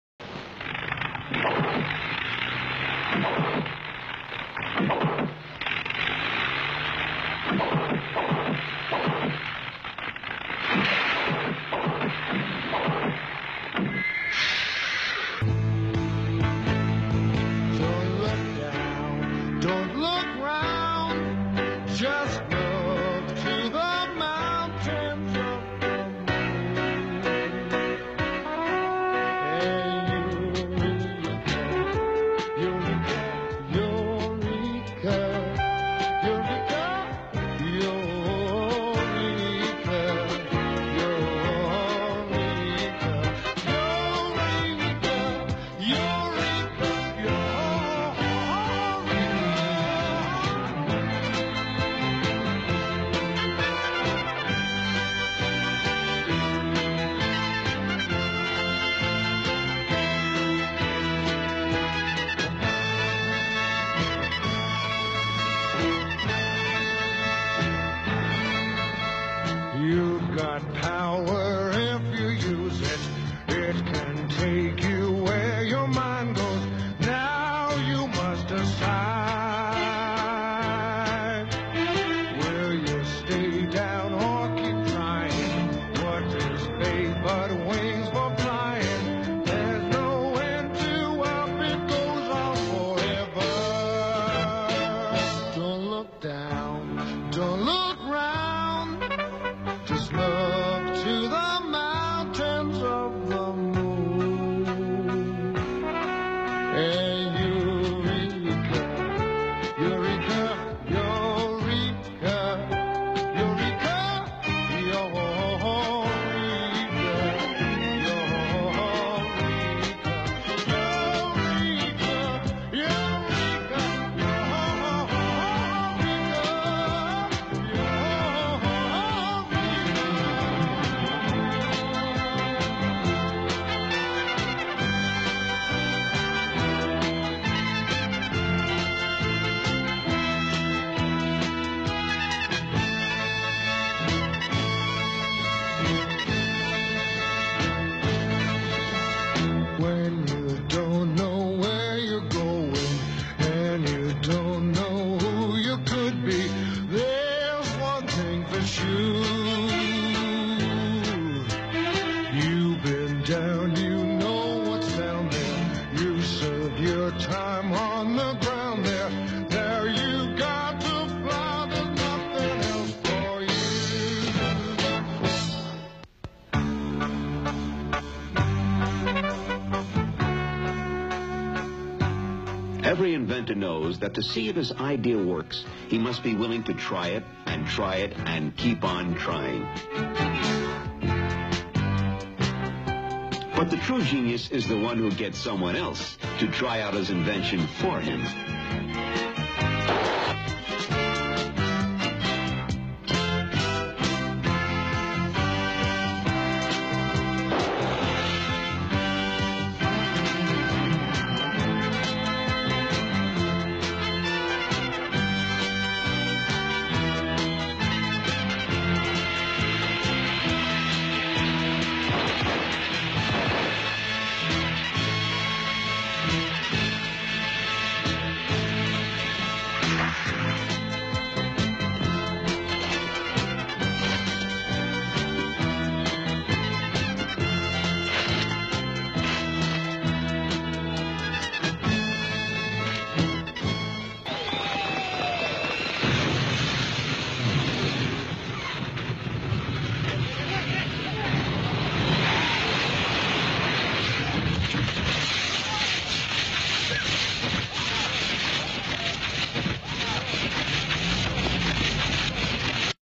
Early flying attempts | first flight| Wright Brothers | Man's
Early flying machines, very old videos when humans were trying to fly in the air and invented many machines. World's Oldest Flying machines, Aeroplane. First Flying Machines - Failures. Early Flying Failures Footage. Orville Wright frist flight